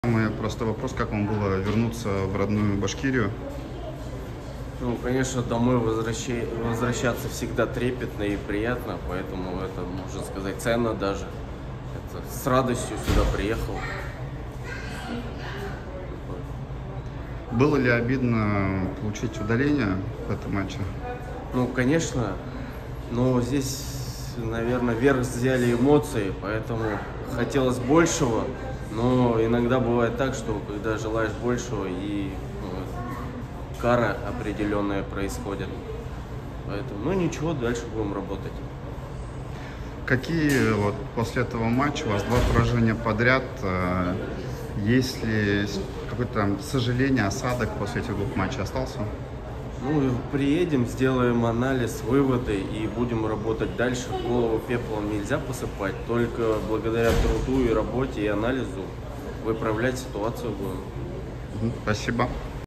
Виктор Курышев: Верх взяли эмоции
-Каково было вернуться в родную Башкирию?

-Домой возвращаться всегда трепетно и приятно, поэтому можно сказать ценно. С радостью сюда приехал.

-Было ли обидно получить удаление в этом матче?

-Ну конечно, но здесь верх взяли эмоции, хотелось большего, но иногда когда желаешь большего происходит определенная кара. Ну ничего, дальше будем работать.

-Остался ли осадок после двух поражений подряд?

-Приедем, сделаем анализ, выводы и будем работать дальше. Голову пеплом нельзя посыпать только благодаря труду и работе, анализу будем выправлять ситуацию.